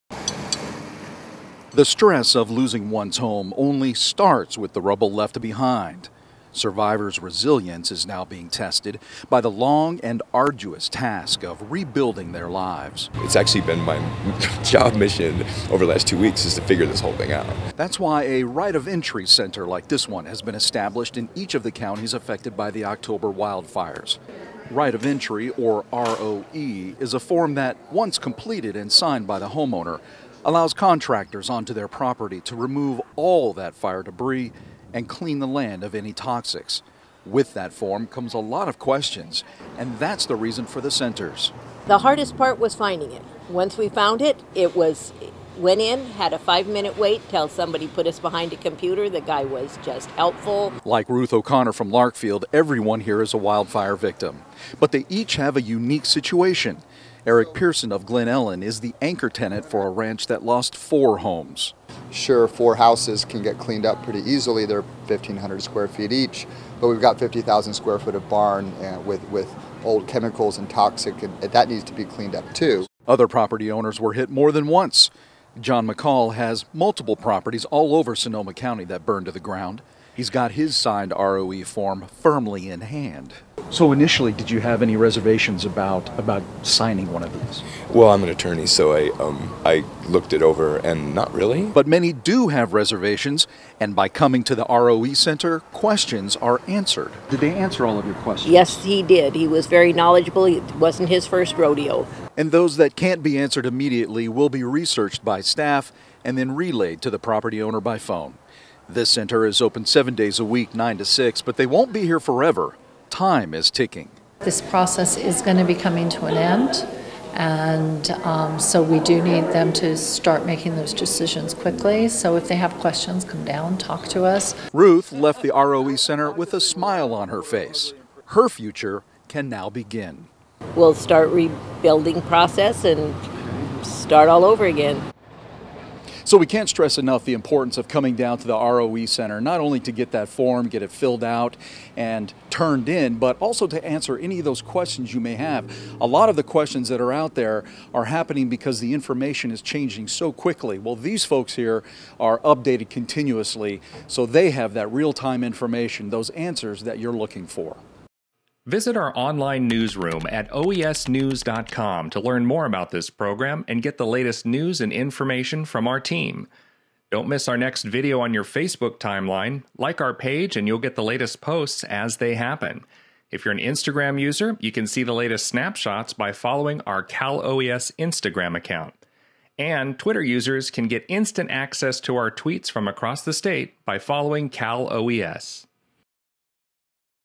Right of Entry Clock is Ticking in Sonoma County
The stress of losing one's home only starts with the rubble left behind.

Survivors' resilience is now being tested by the long and arduous task of rebuilding their lives.

John McCaull, a resident of Glen Ellen says, "it's actually been my job mission over the last two weeks to figure this whole thing out."

That's why a "right of entry" center like this one has been established in each of the counties affected by the October wildfires.

"Right of entry," or ROE, is a form that once completed and signed by the homeowner allows contractors onto their property to remove all that fire debris, and clean the land of any toxics.

With that form comes a lot of questions and that's the reason for the centers.

"The hardest part was finding it,” says Ruth O’Connor, who lost her home in Larkfield. “Once we found it, went in had a five-minute wait until somebody put us behind a computer the guy was just helpful."

Like Ruth, everyone here is a wildfire victim.
But they each have a unique situation.
Eric Pearson of Glen Ellen is the anchor tenant for a ranch that lost 4 homes.

"Sure, four houses can get cleaned up pretty easily. They're 1500 square feet each. But we've got 50,000 square feet of barn with old chemicals and toxic chemicals, that needs to be cleaned up too."

Other property owners were hit more than once.
John McCaull has multiple properties all over Sonoma county that burned to the ground.
He's got his signed ROE form firmly in hand.

"So initially did you have any reservations about signing one of these?” we asked him. He responded “Well, I'm an attorney so I looked it over and not really."

But many do have reservations and by coming to the roe center questions are answered.

"Ruth, did they answer all of your questions? Yes, he did. He was very knowledgeable. It wasn't his first rodeo," said Ruth.

And those that can't be answered immediately will be researched by staff and then relayed to the property owner by phone.
This center is open 7-days a week, 9-6, but they won't be here forever; time is ticking.

"This process is going to be coming to an end so we do need them to start making those decisions quickly,” urged Christine Sosko, Director of Environmental Health for Sonoma County. “So if they have questions come down talk to us."

Ruth left the ROE center with a smile on her face; her future can now begin.
"We'll start rebuilding process and start all over again."

"So we can't stress enough the importance of coming down to the ROE center, not only to get that form, get it filled out and turned in, but also to answer any of those questions you may have,” says Shawn Boyd, public information officer for Cal OES. “A lot of the questions that are out there are happening because the information is changing so quickly. well, these folks here are updated continuously so they have that real-time information, those answers that you're looking for."